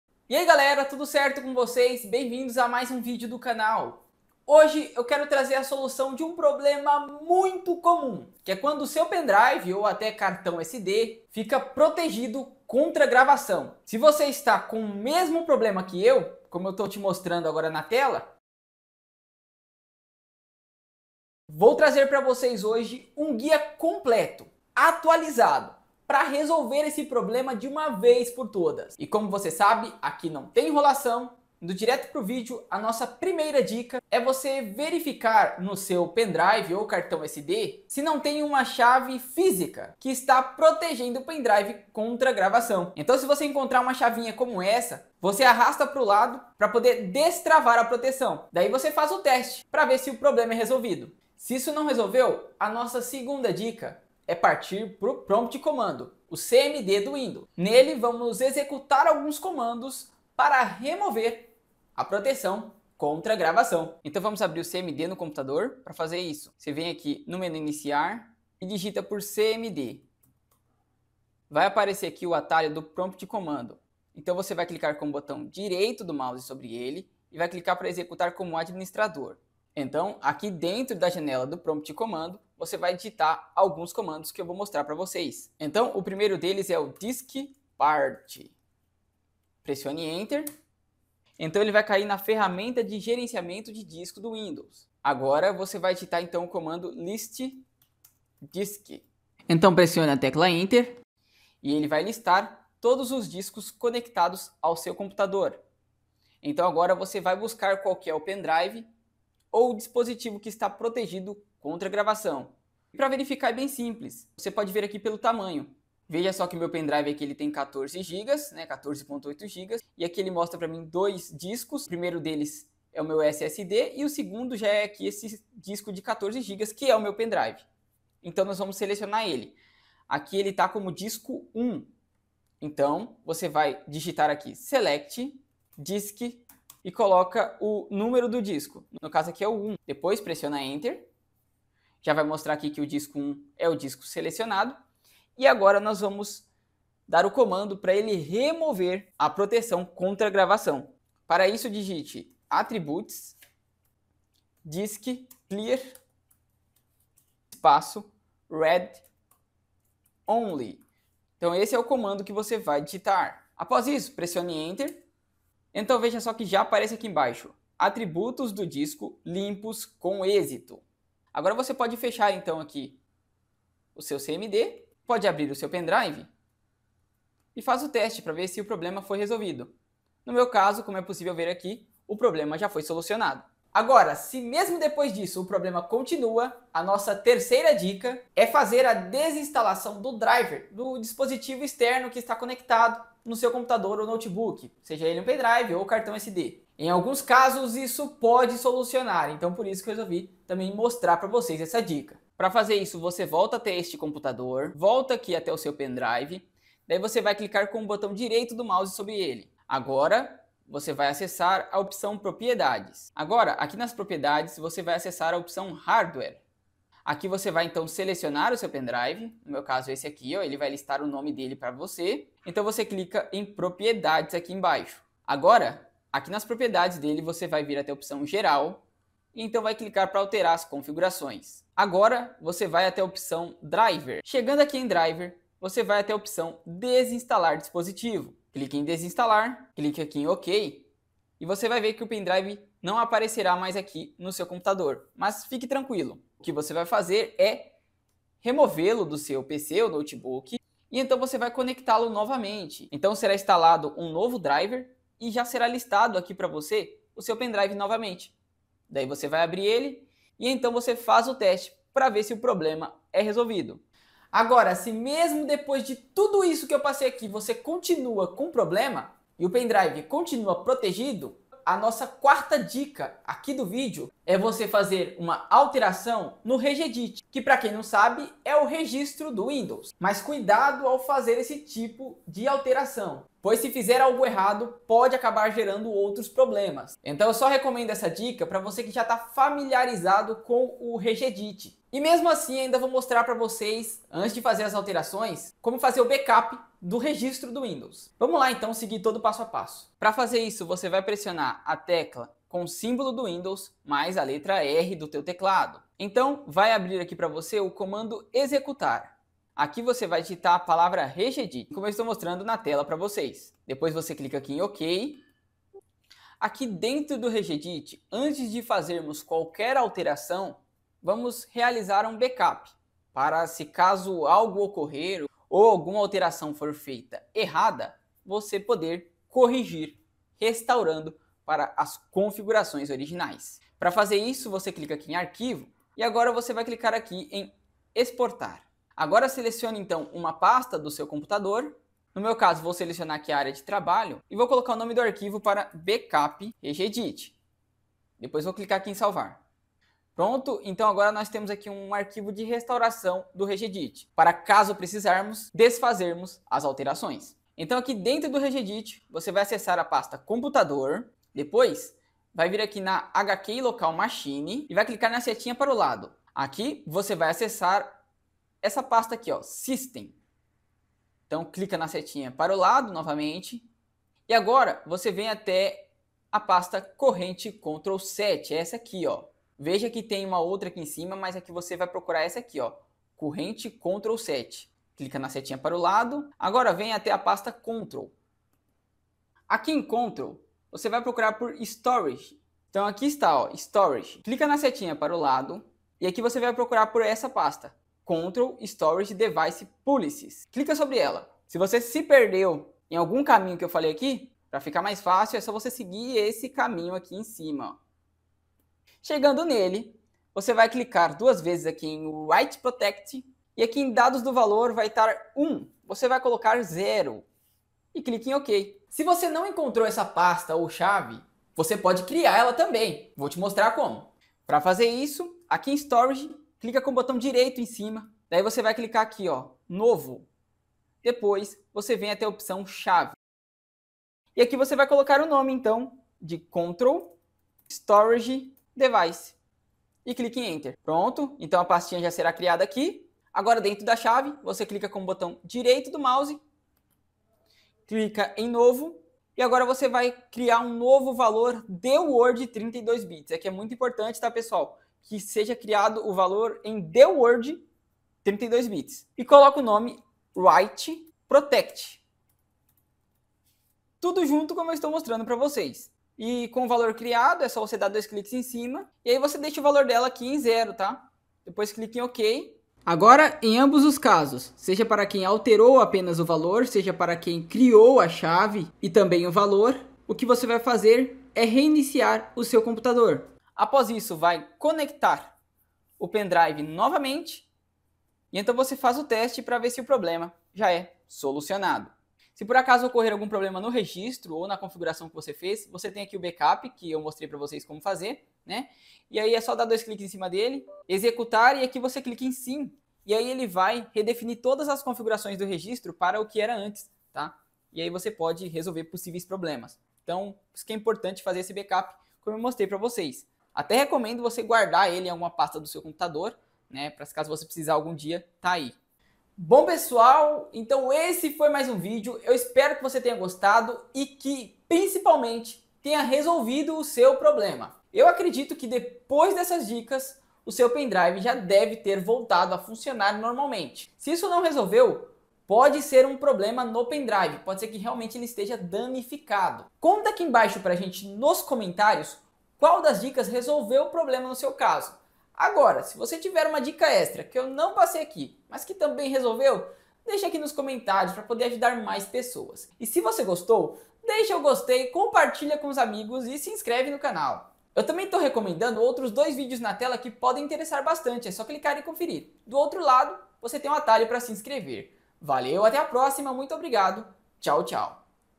Como REMOVER PROTEÇÃO CONTRA GRAVAÇÃO DO PENDRIVE ou CARTÃO SD!
Não consegue transferir nenhum arquivo (seja foto, vídeo ou documento e etc) para seu Pendrive e mostra que o dispositivo está protegido contra gravação e você não sabe o que fazer? Neste vídeo quero te mostrar um guia completo e detalhado para você remover a proteção contra gravação de um Pendrive ou Cartão SD e voltar a transferir os arquivos e usar seu dispositivo normalmente. Tire todas as suas dúvidas em mais um vídeo!